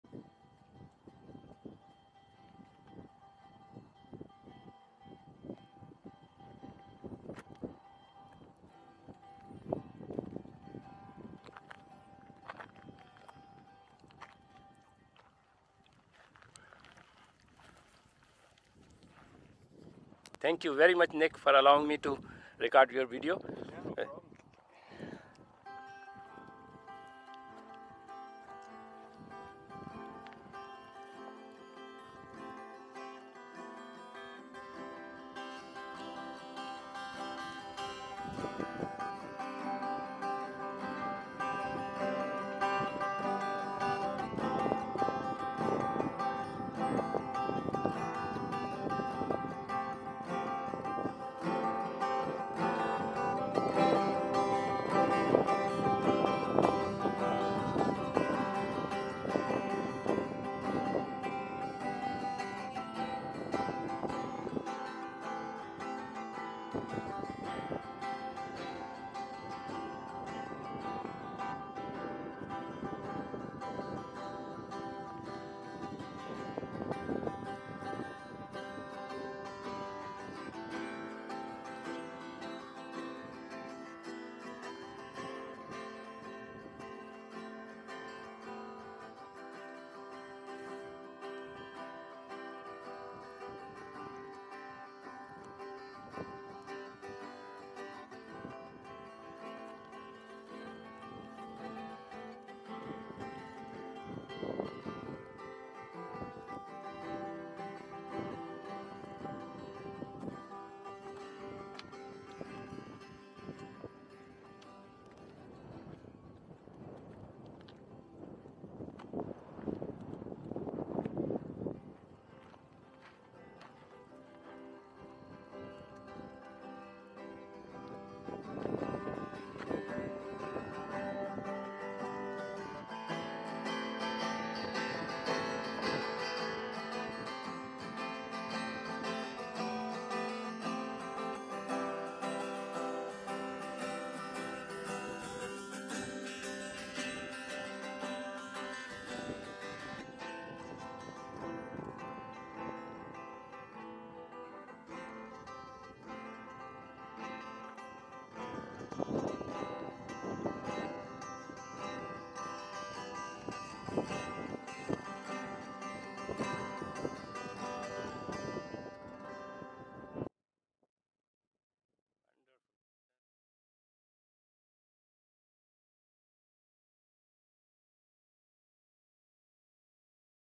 Nick and his guitar
Nick with his guitar in Echodale Regional Park.
Medicine Hat, AB Canada. Aug 01 2021.